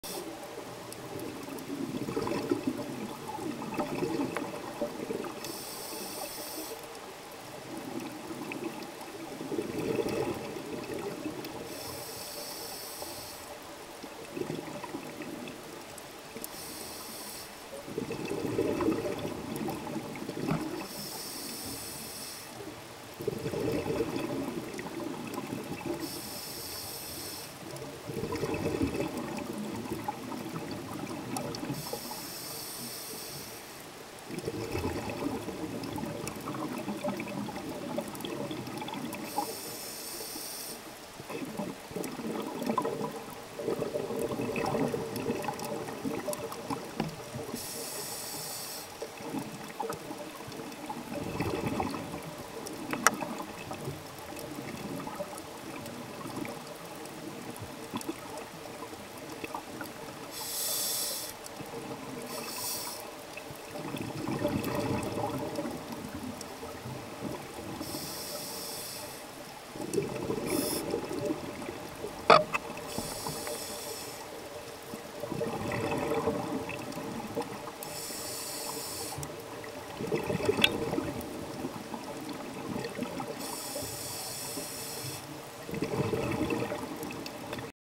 Roatan-tangs-n-surgeonfish
Nice school of fish in Blue channel dive site - Roatan